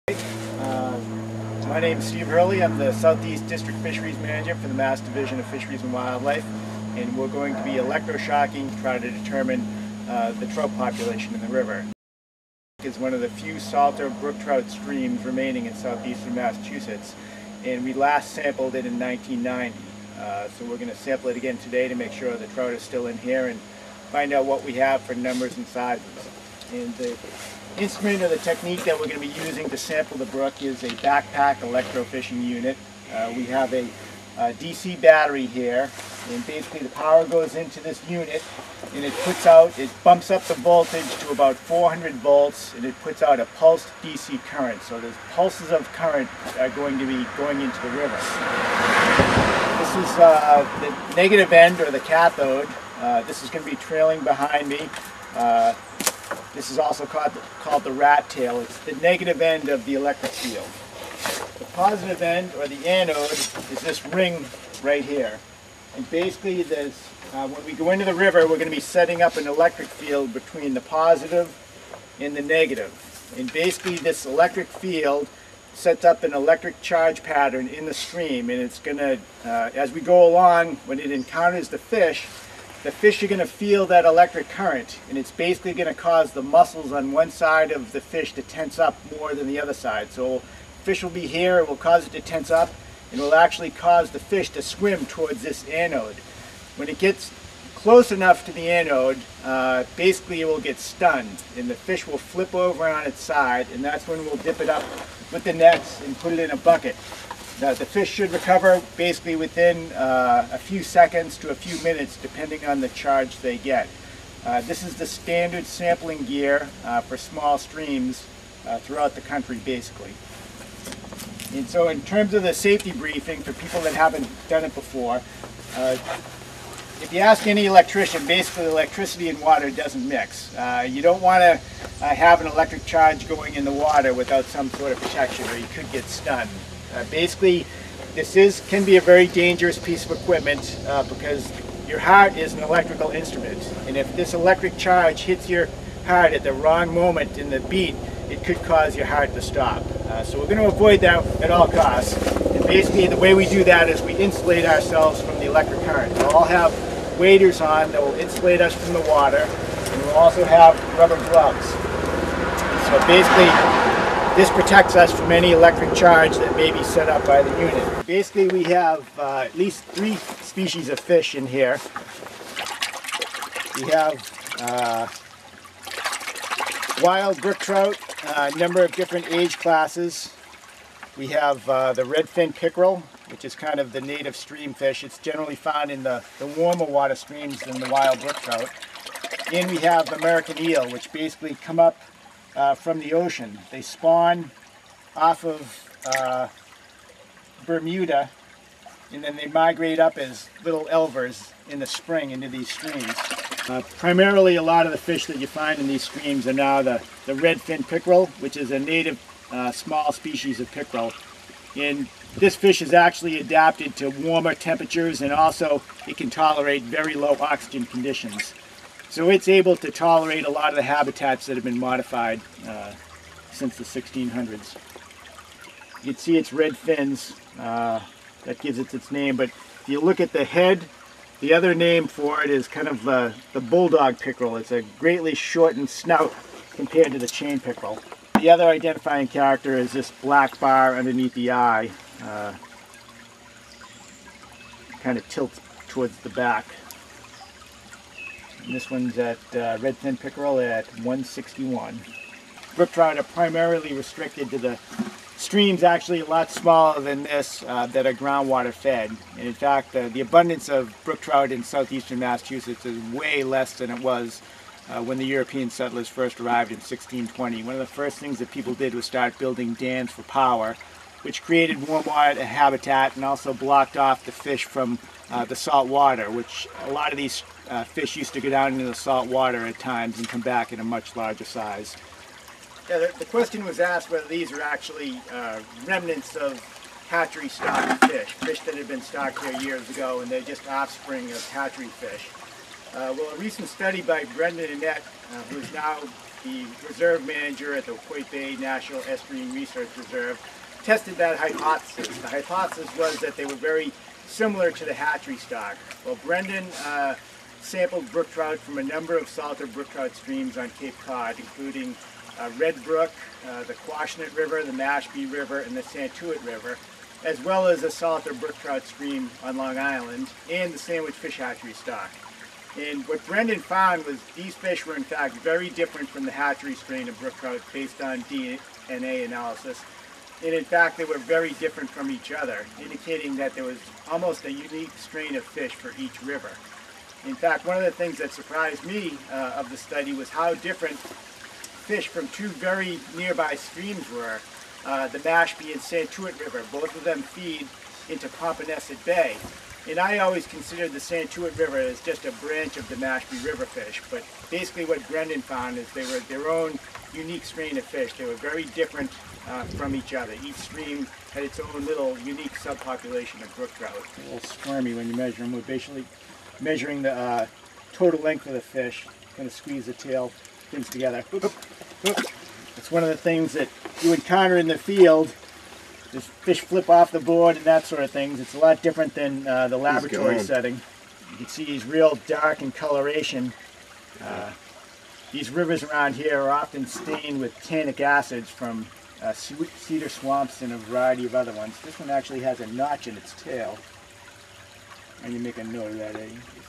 Electroshocking Spring 2007 Unnamed Location SE Massachusetts
View Mass Fish and Wildlife (aided by volunteers) doing electroshocking of a trout stream that contains salter brooktrout and hear biologist Steve Hurley talk about electroshocking and trout.